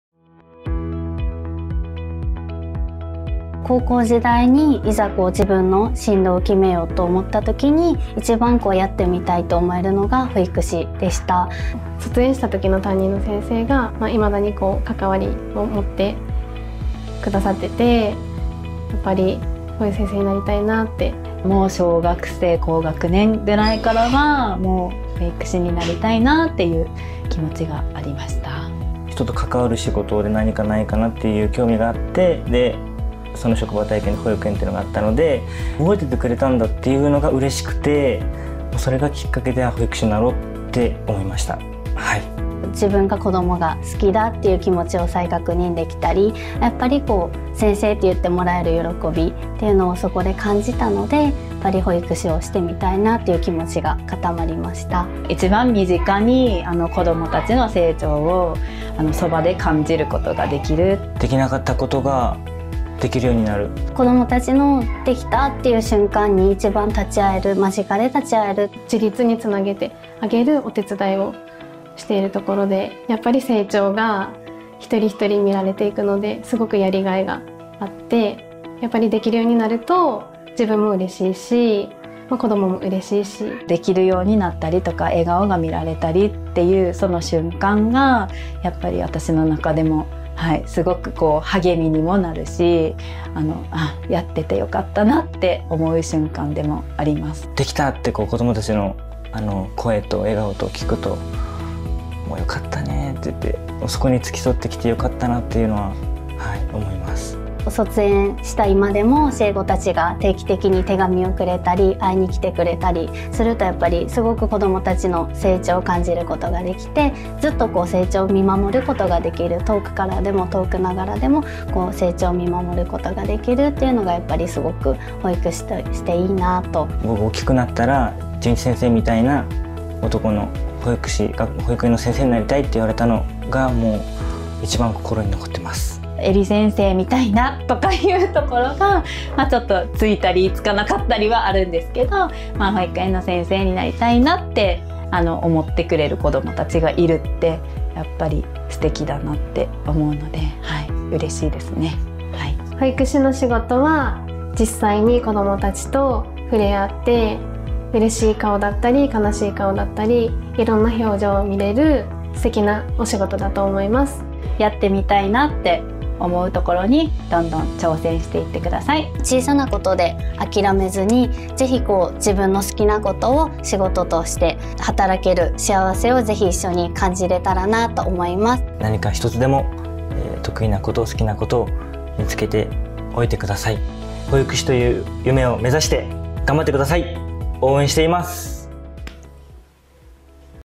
将来、保育の現場で働きたい皆さんへ！～保育士等の仕事の魅力（ダイジェスト版）～
学生向けに保育士等の仕事の魅力をお届けします。